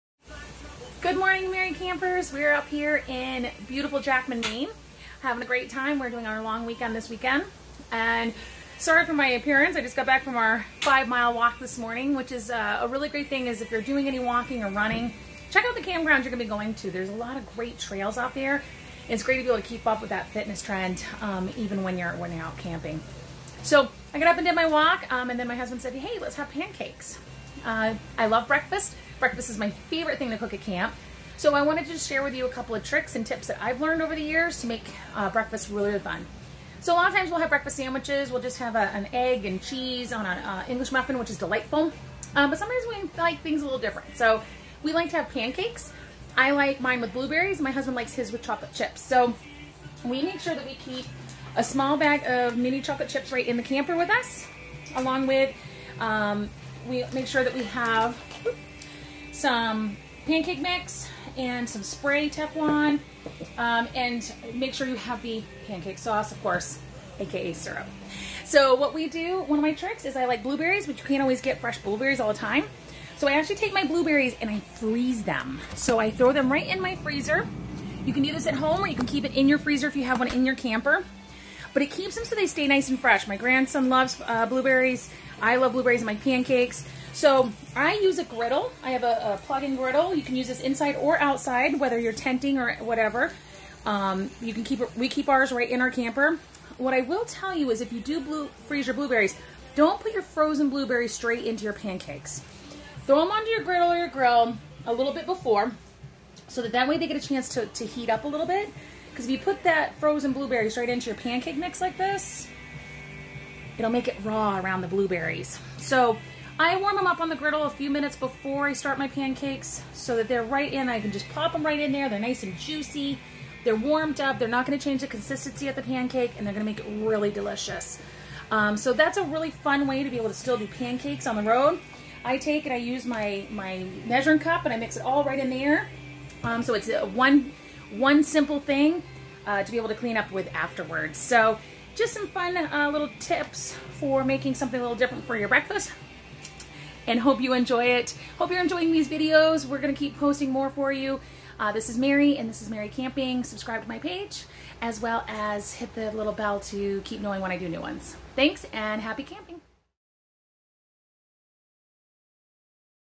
Merry Camping and Breakfast in the BIG RIG
Breakfast. It has been said it is the most important meal of the day.  Check out a quick little video of how we start the day with a good meal